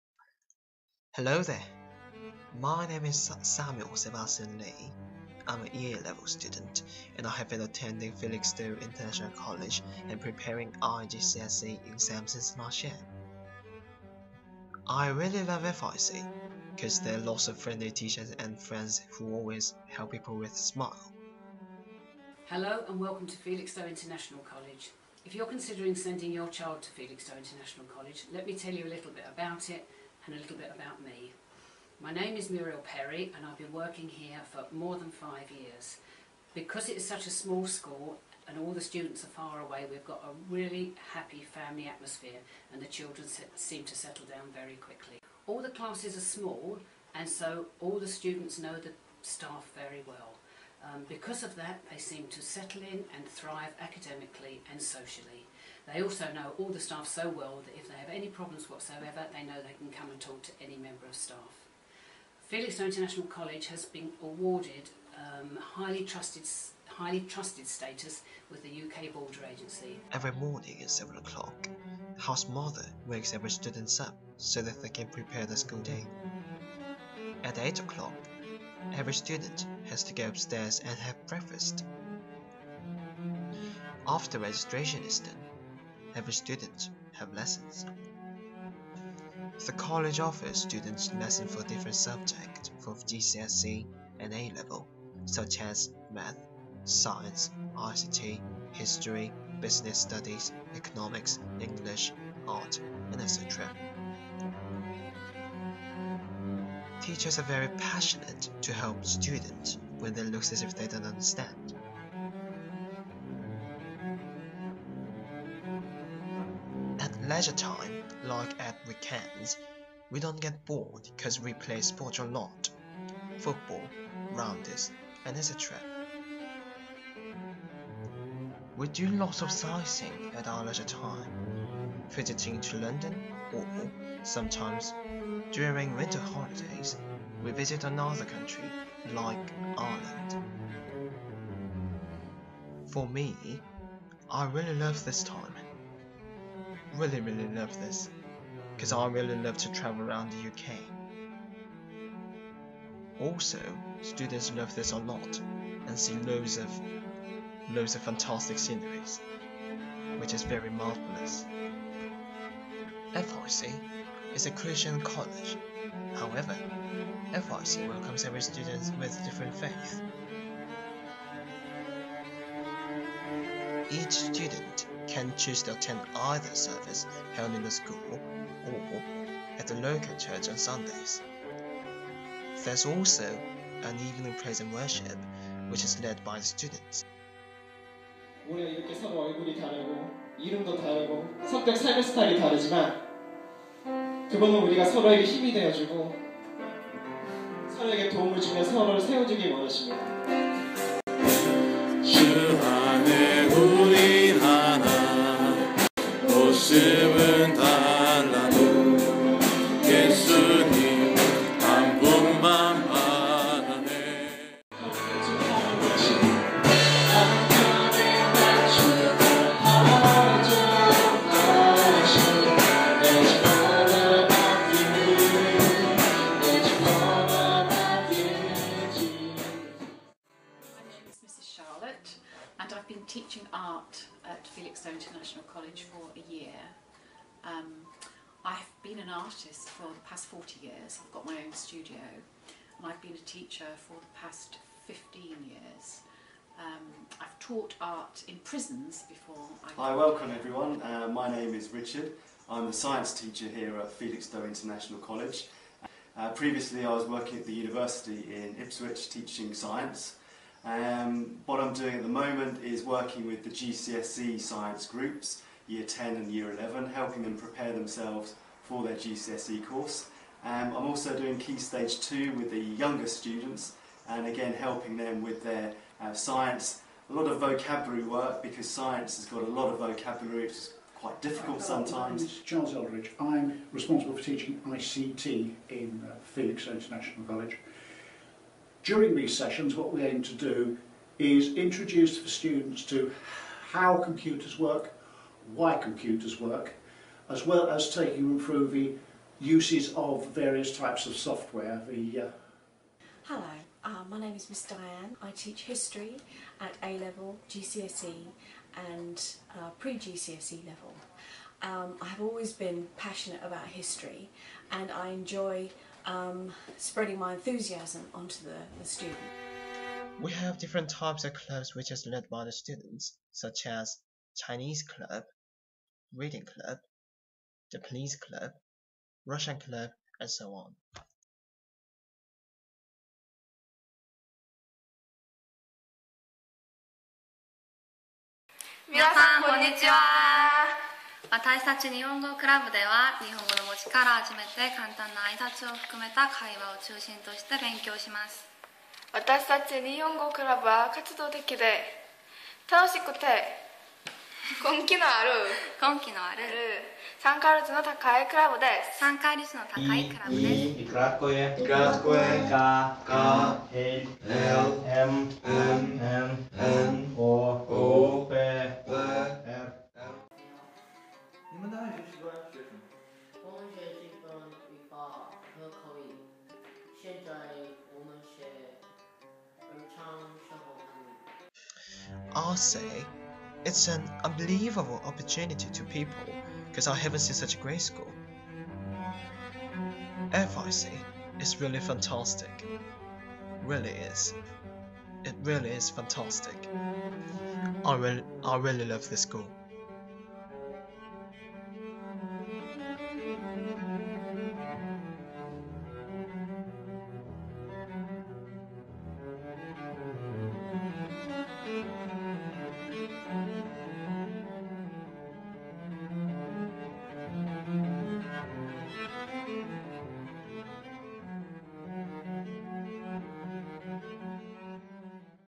영국 재학생의 학교자랑 - I really really love FIC!
The video was made by Samuel Sebastian Lee who have studied in the Felixstowe International College in UK for a year. He talks about classes, lessons, clubs, games, activities, trips and more.

이 비디오는 영국 FIC에서 1년동안 공부해온 재학생 이 사무엘 학생이 만든것입니다.
사무엘학생은 이 비디오에서 학급들, 수업들, 클럽, 엑티비티, 여행 등등 많은것을 설명합니다..